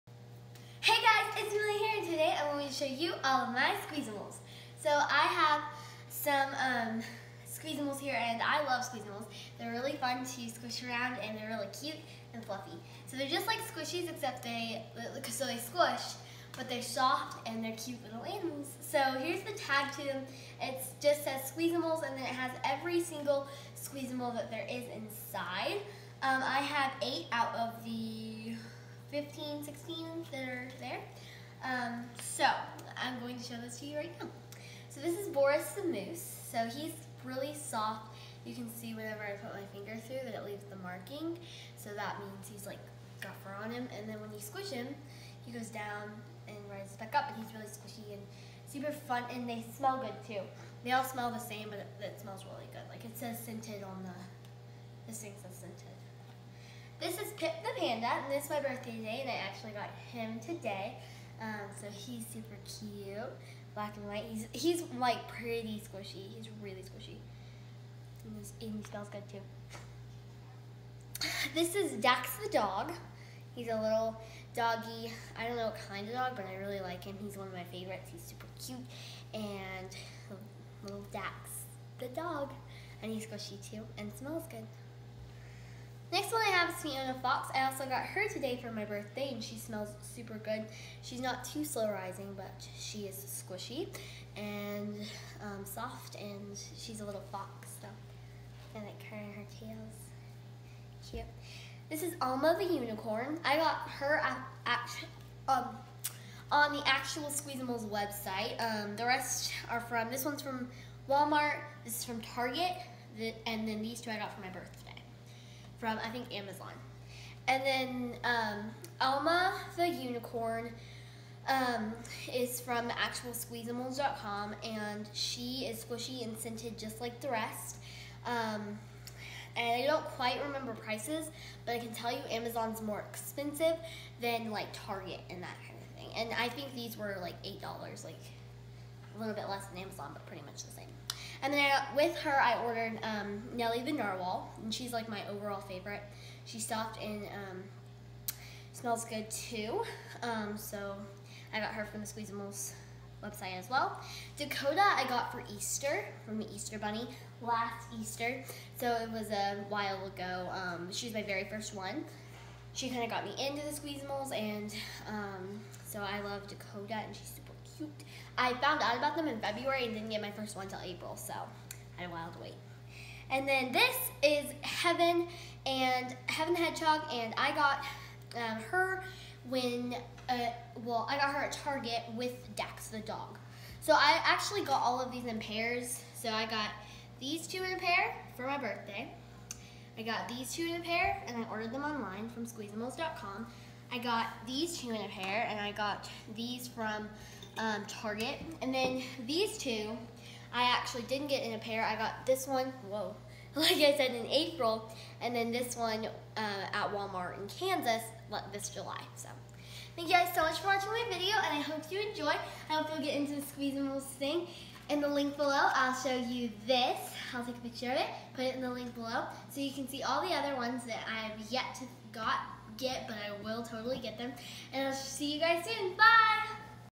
Squishamals Review
Amelia reviews squishamals aka ‘squeezamals’